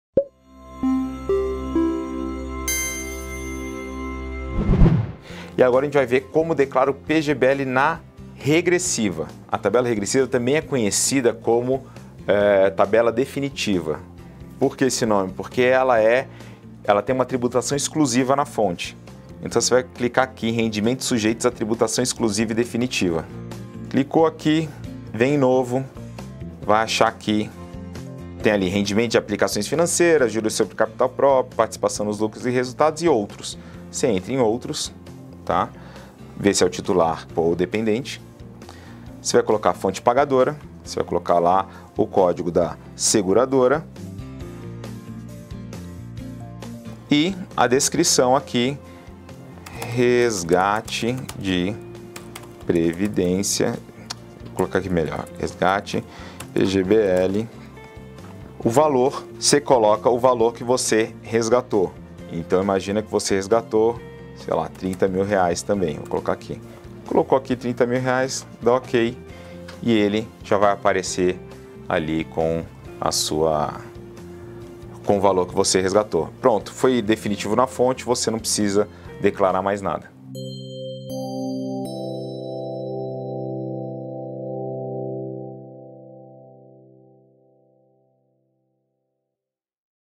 #04 DECLARAR RESGATE PGBL TABELA REGRESSIVA
PGBL: Como declarar previdência previda no imposto de renda 2019! Confira nosso eBook completo e não perca nenhum detalhe.